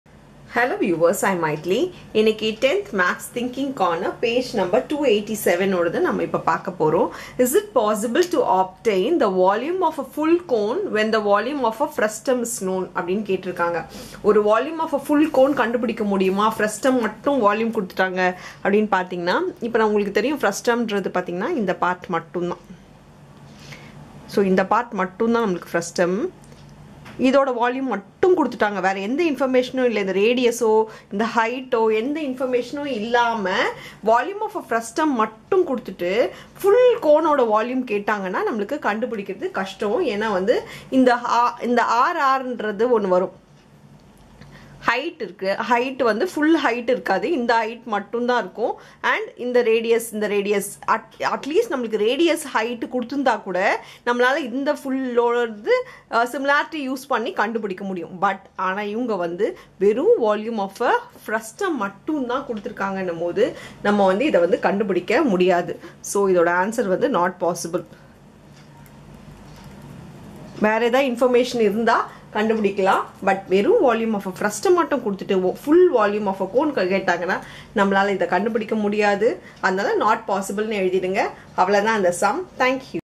TN 10 Maths Mensuration Thinking Corner Page 287
Tamil Nadu State Board Samacheer 10 Maths Complete Solution For Thinking corner Page 287 In Mensuration